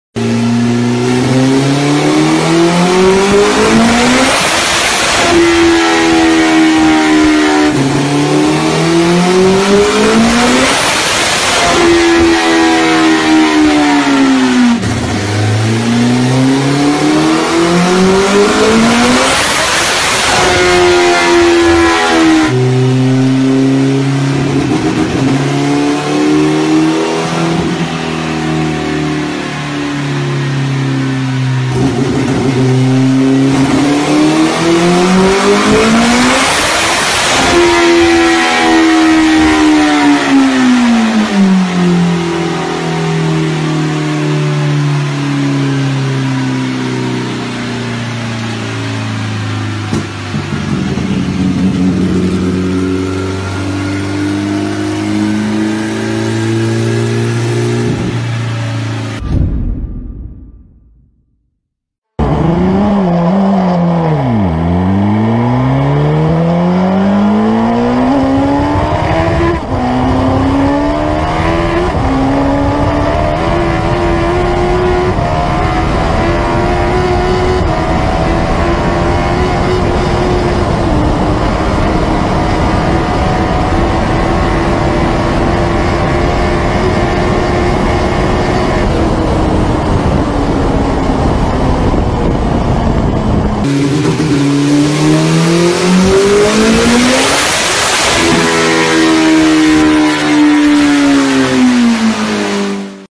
900+HP HAYABUSA TURBO
900+HP HAYABUSA TURBO
A bike with the same power as a Bugatti Veyron
Big CC Racing Turbo Hayabusa otherwise known as 'Project Pisstake' makes over 900 horsepower at the rear wheel. This is easily well over 1000hp at the crankshaft. Put into context a Bugatti Veyron makes 1000 at the crankshaft with 16 cylinders & 4 turbochargers involved in producing it. The Suzuki Hayabusa has 4 cylinders & this engine is modified to 1.5 litre. A Veyron is 8 litre.
credit: @Big CC Racing @Sean Mills